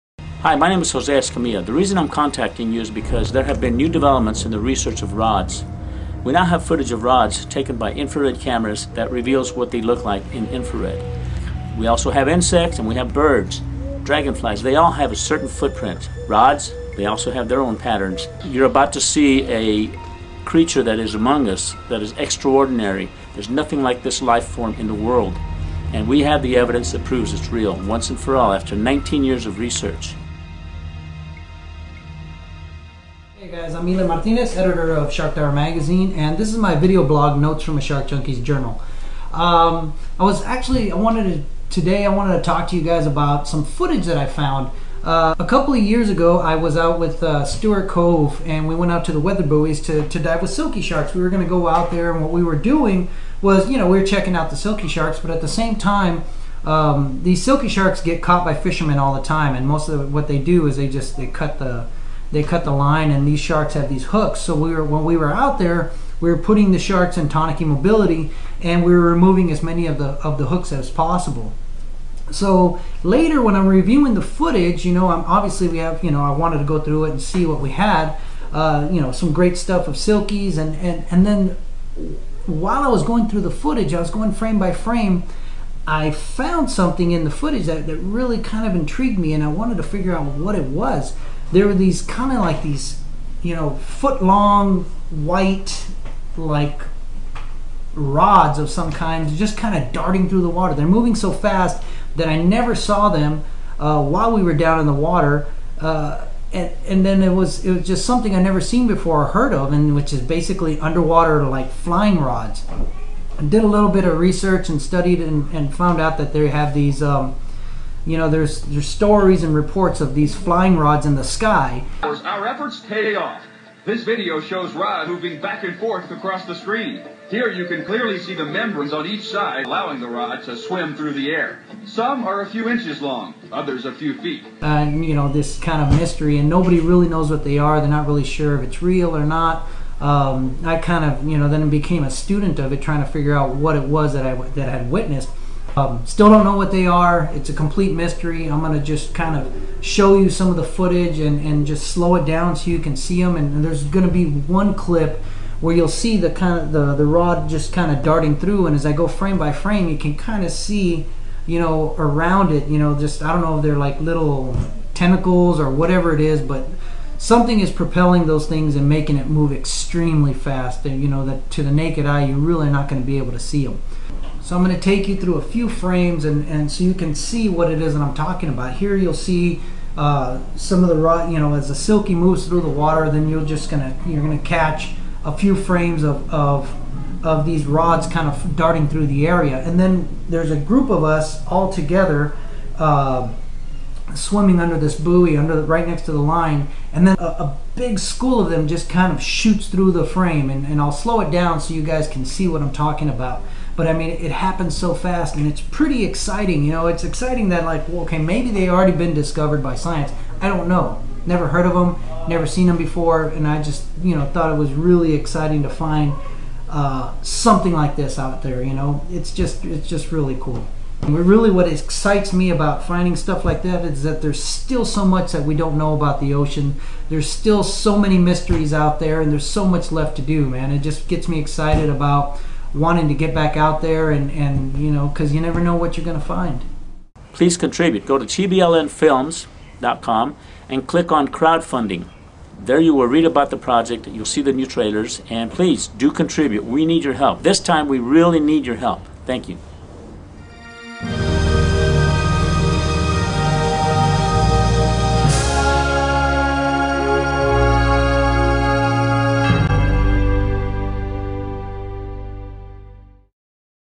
Roswell Rods Breaking News
Underwater RODS footage Courtesy of: Eli Martinez - SDMadventures.com - Shark Diver Magazine.